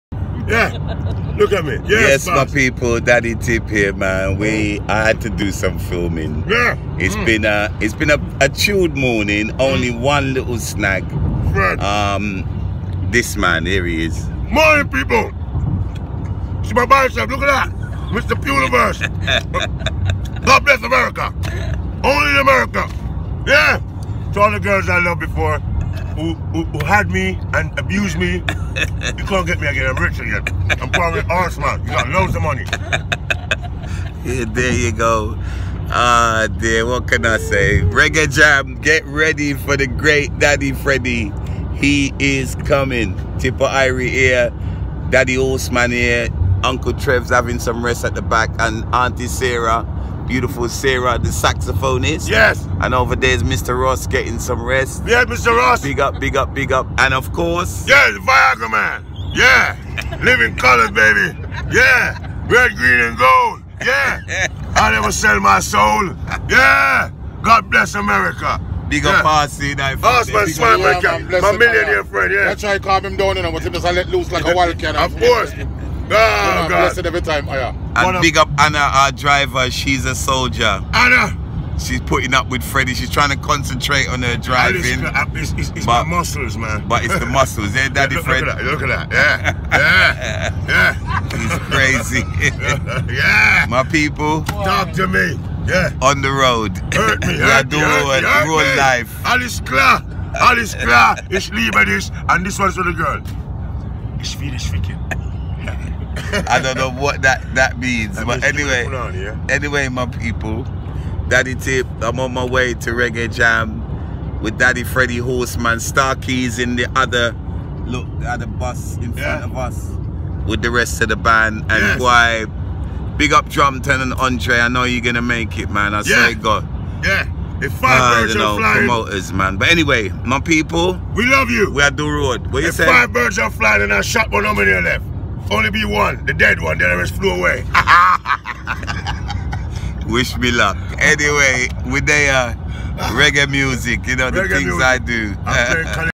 Tippatv in the rd with Daddy Freddy
Tippa Irie & Daddy Freddie in Germany 2018 reggae jam